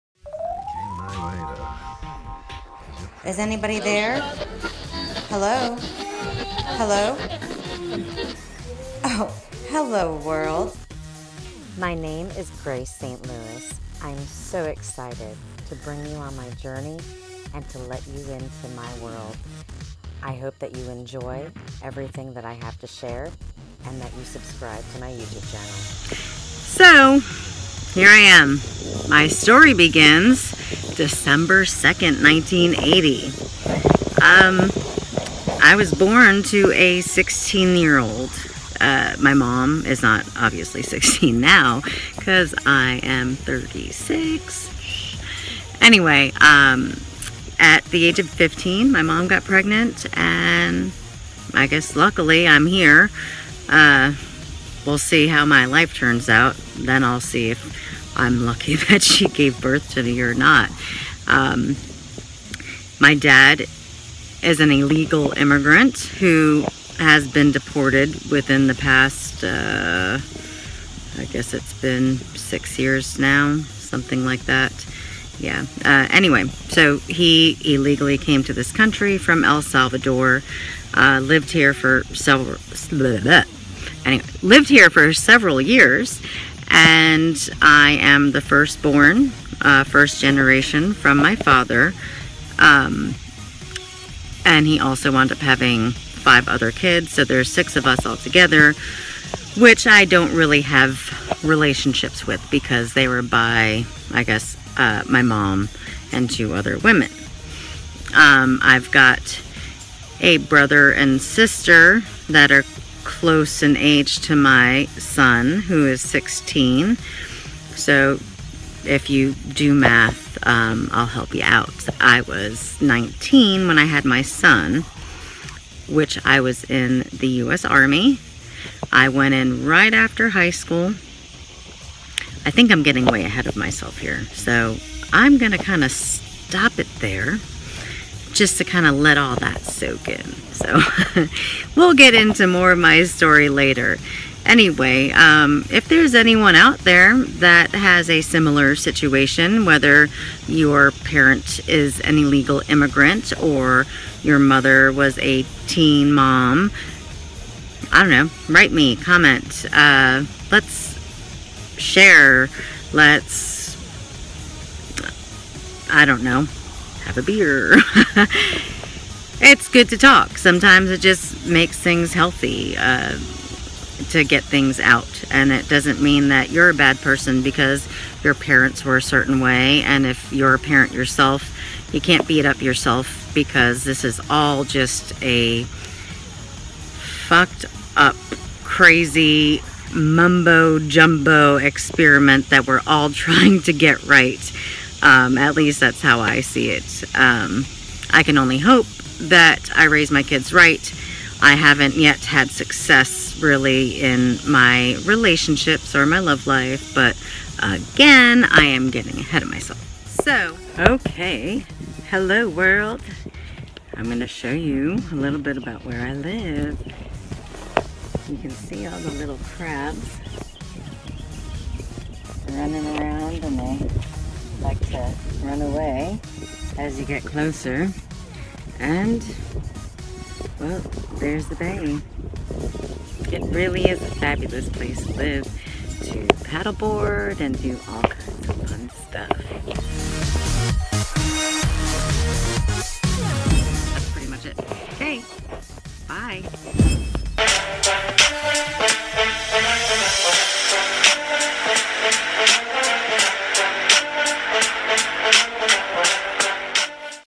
Intro to journey through life with Grace St. Louis
Sharing my story with those wanting to join me on this crazy ride. It's been bumpy this far but let's see what this funked up ride has in store.